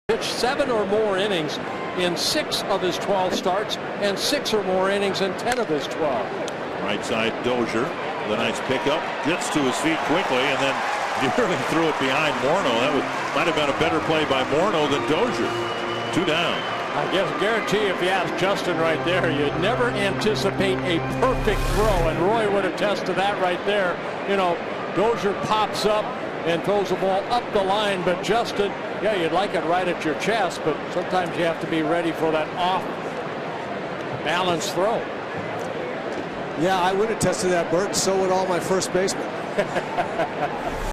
HOU@MIN: Dozier robs Krauss with slide at second
8/2/13: Brian Dozier slides to his left to reach a ground ball to second, spinning and throwing to first to retire Marc Krauss